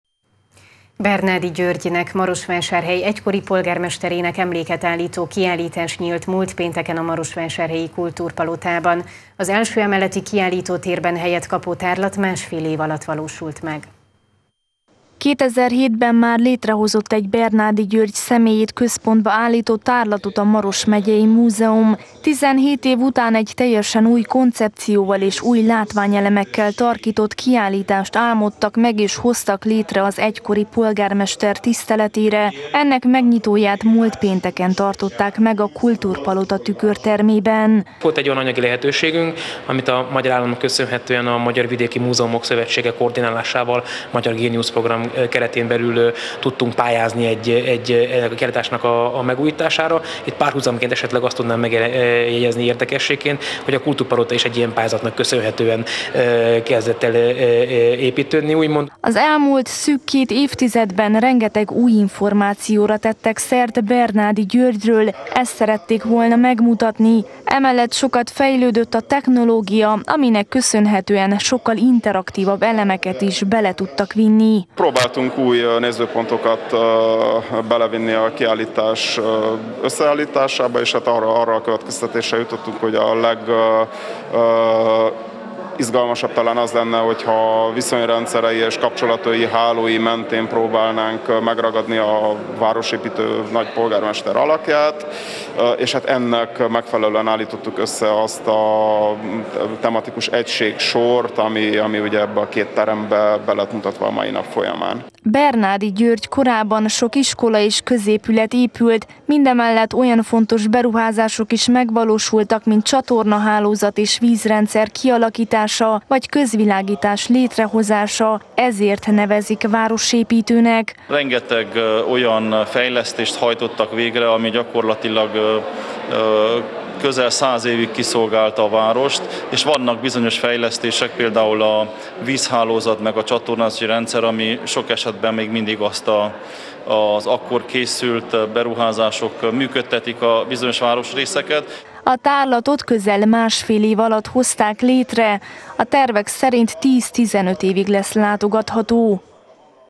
Bernády György-kiállítás új koncepcióval – Erdélyi Magyar Televízió
Bernády Györgynek, Marosvásárhely egykori polgármesterének emléket állító kiállítás nyílt múlt pénteken a marosvásárhelyi kultúrpalotában. Az első emeleti kiállítótérben helyet kapó tárlat másfél év alatt valósult meg.